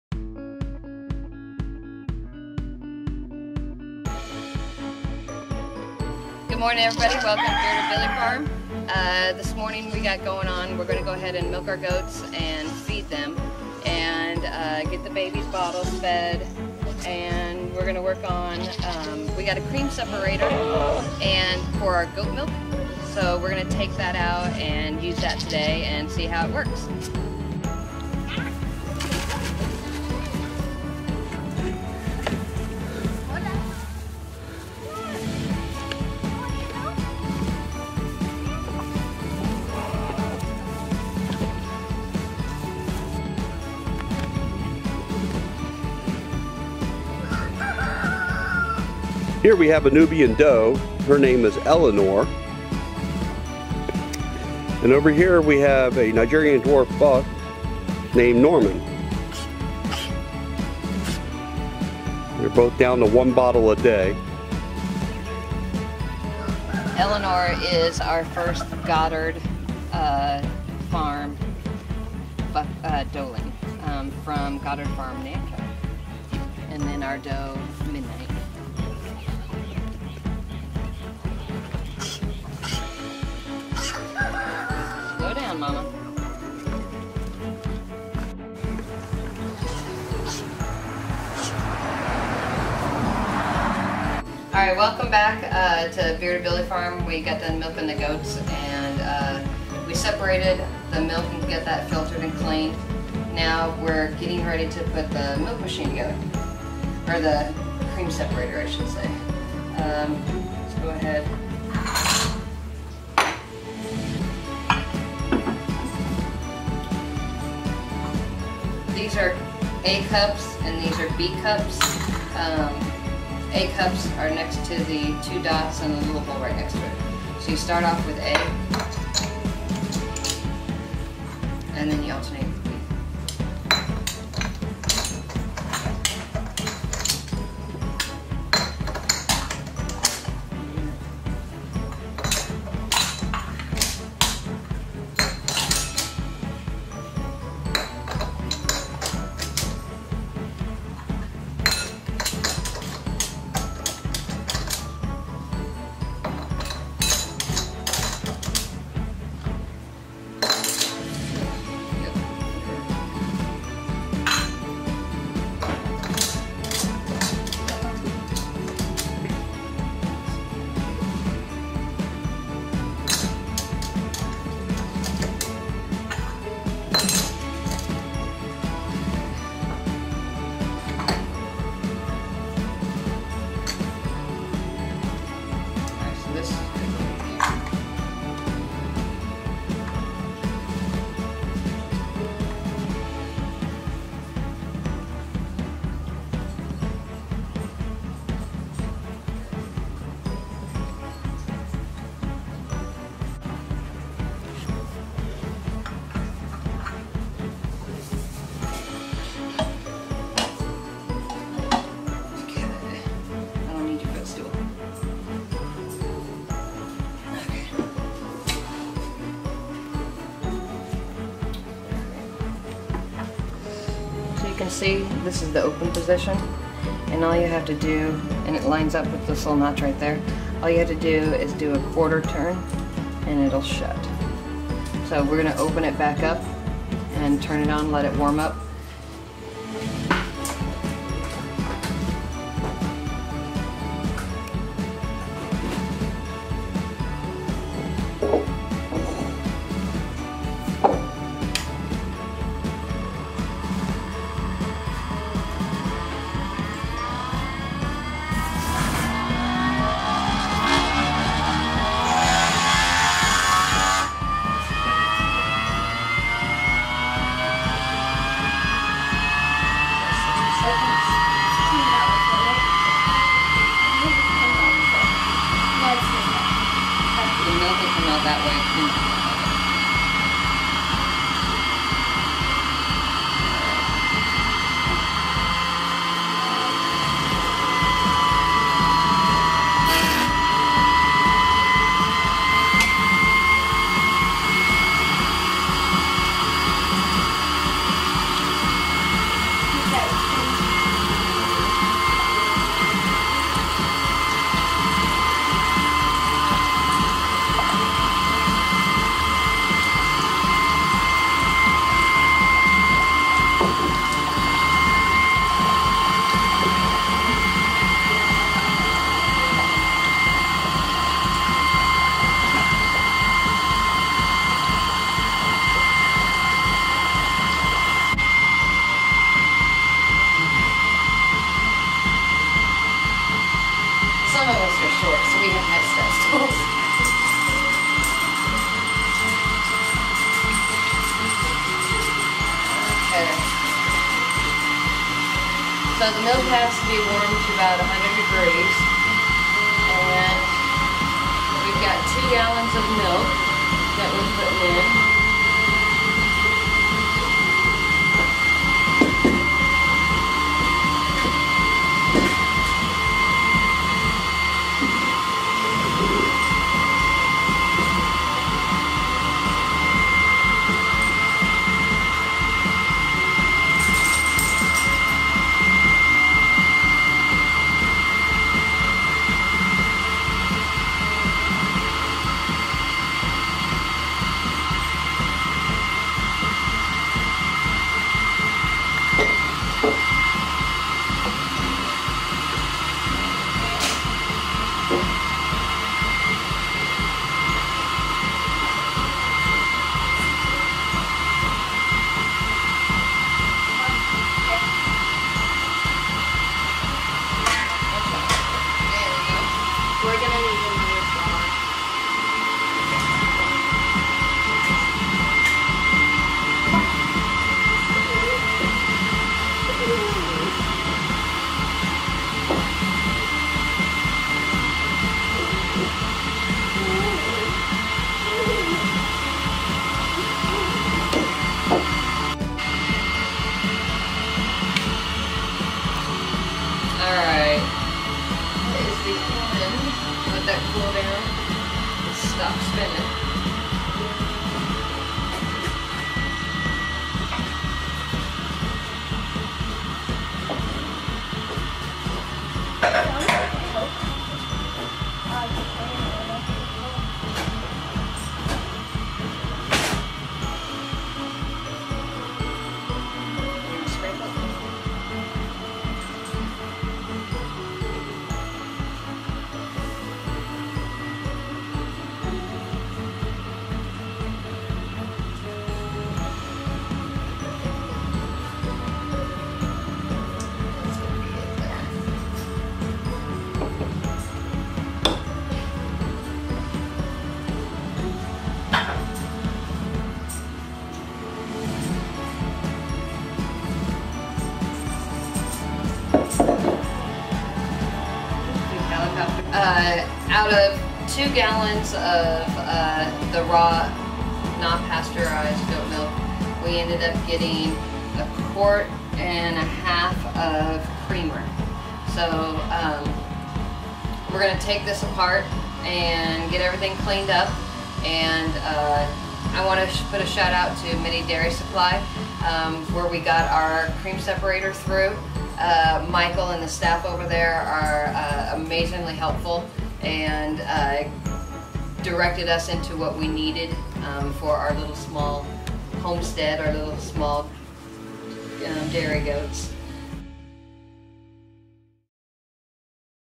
Amanda shows how easy is to use model 100-18 cream separator
Amanda from Bearded Billy Farms shows how fun and easy is to use Motor Sich Cream Separator Model 100-18.